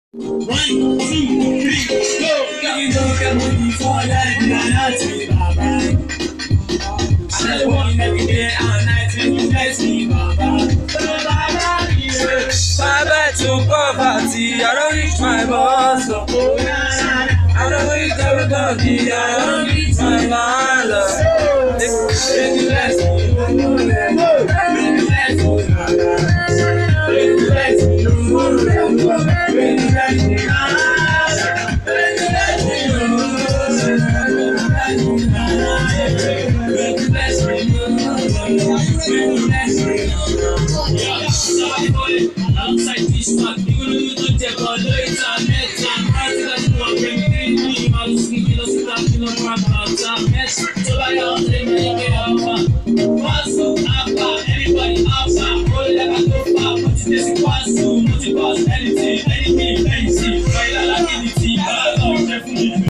Gee-swag perform @ Lafiesta 2018 kwara hotel Ilorin kwara state
Unsigned